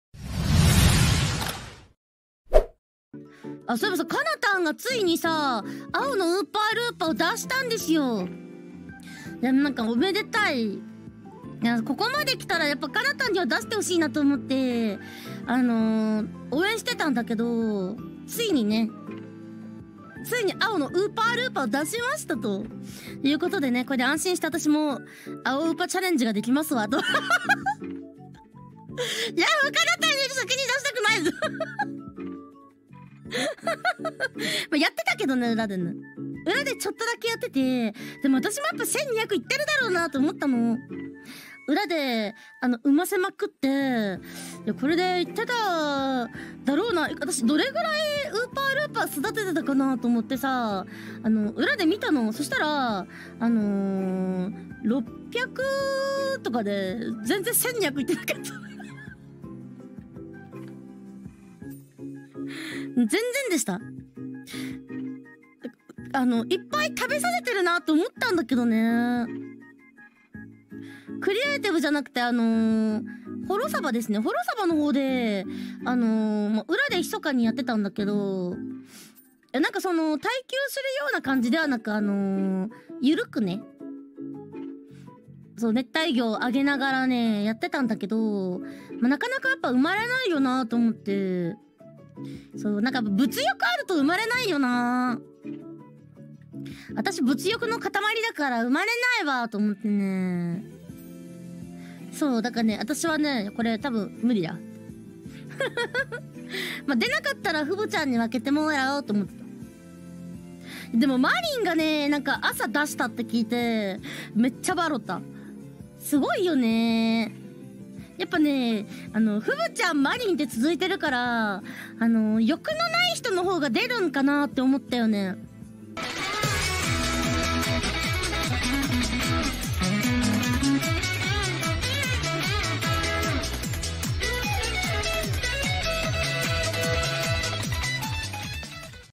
Flare Is Happy That Kanata Get Her Blue Axolotl Despite Flare Breeding Them First【Hololive Eng Sub】
Hololive Member